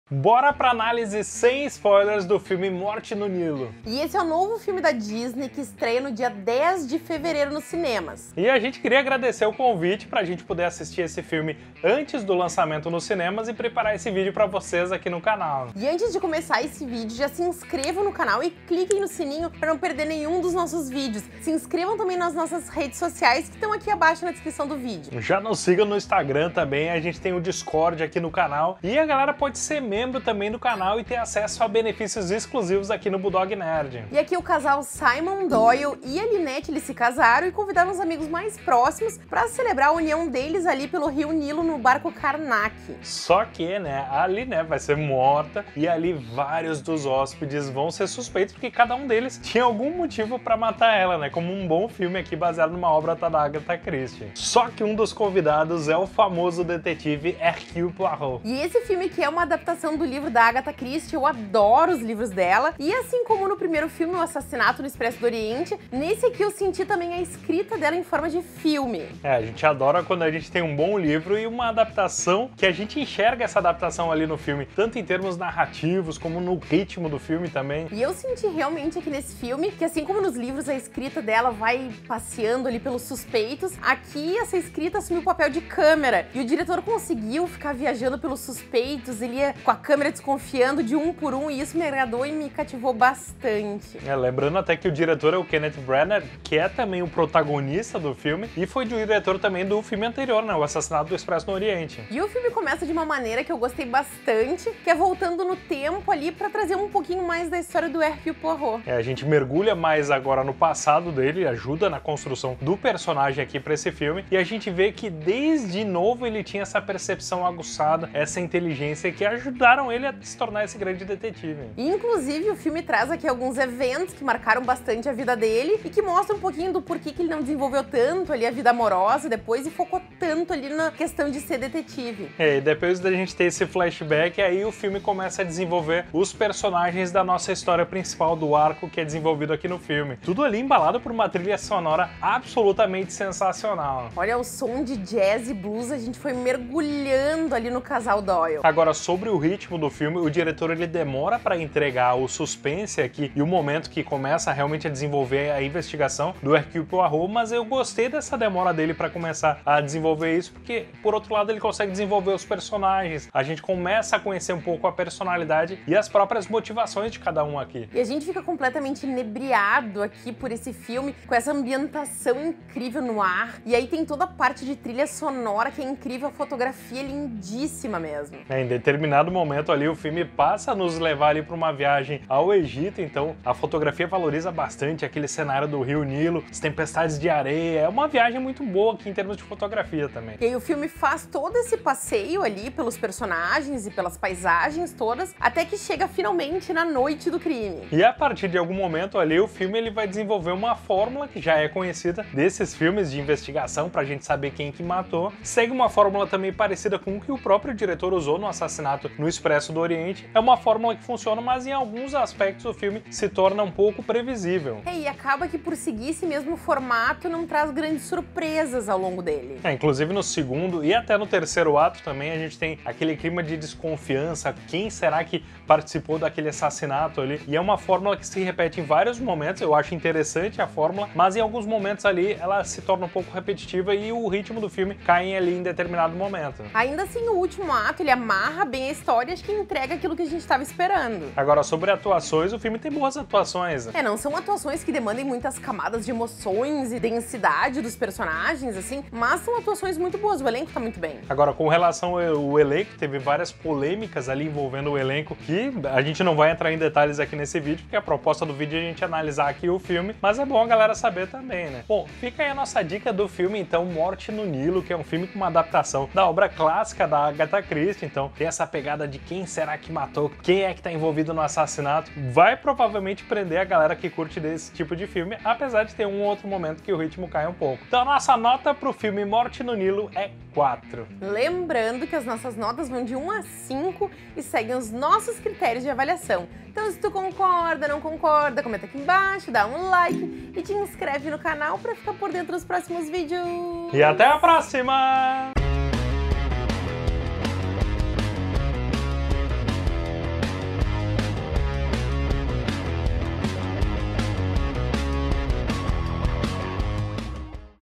MORTE NO NILO | Vale a pena assistir? | análise sem spoilers
Análise sem spoilers sobre o filme "Morte no Nilo".

Gamertag: BulldogNerdXbox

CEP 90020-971
Porto Alegre-RS